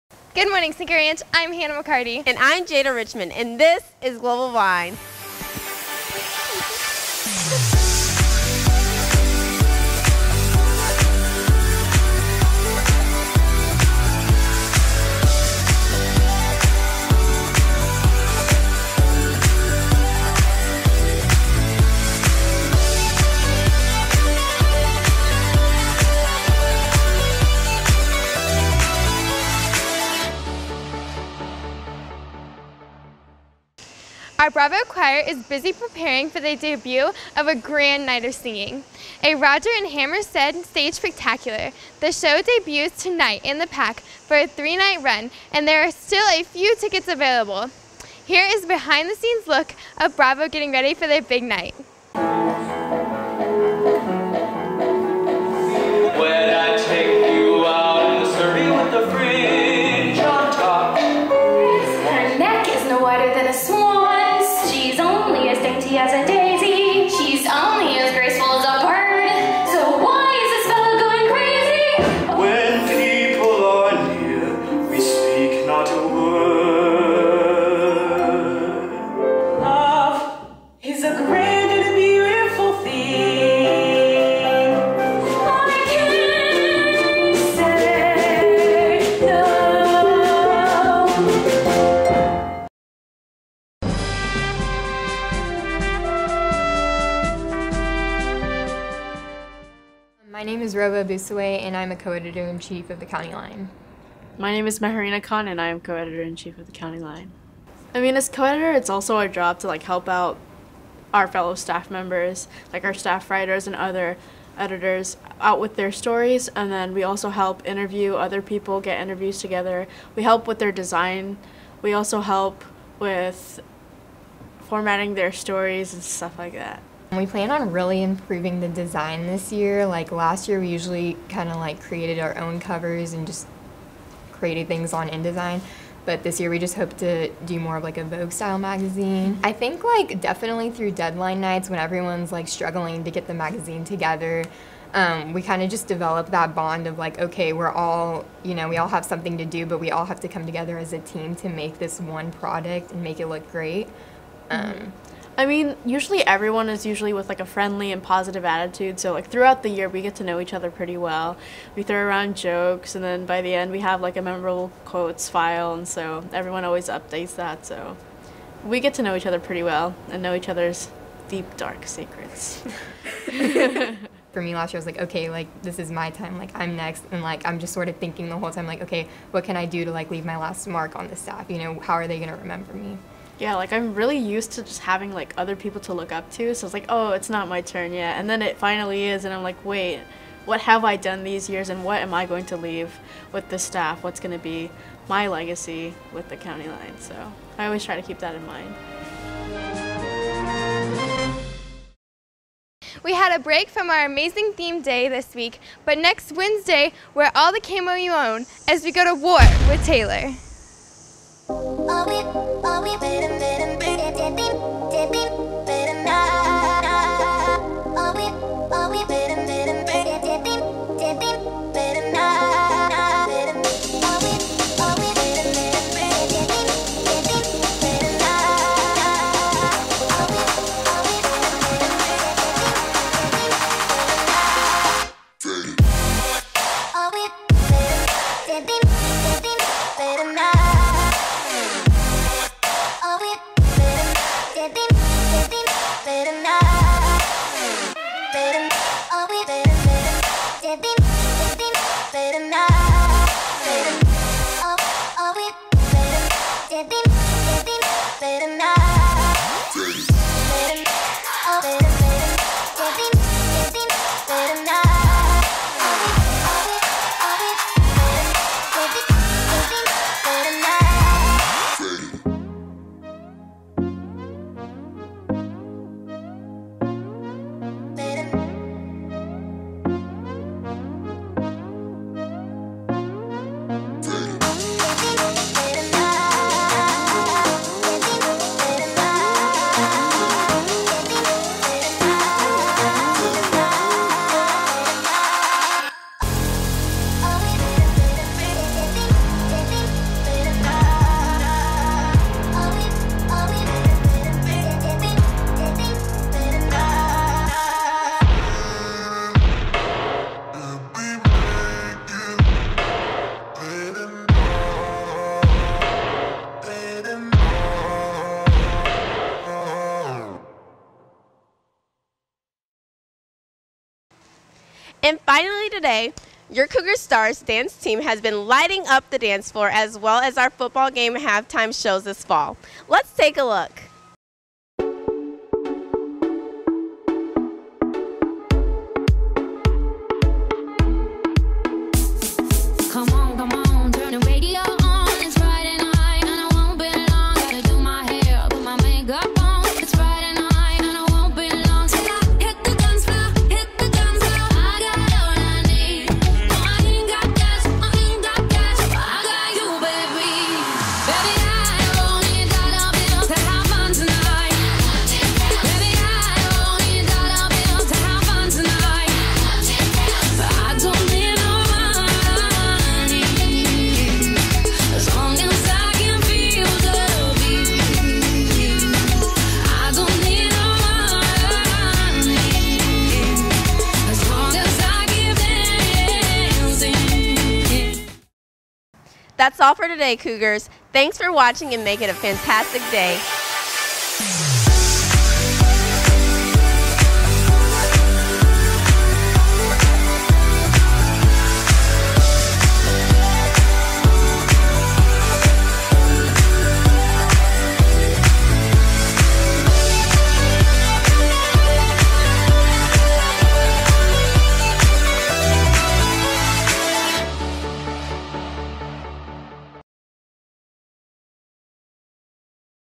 September 29 - Global Vine. Cinco Ranch High School
Set to 1080pHD for best results

Featuring segments on the Cougar STARS Dance Team, The Bravo Show Choir, the Cougar Varsity Football team and the County Line Newspaper

Global Vine. Cinco Ranch High School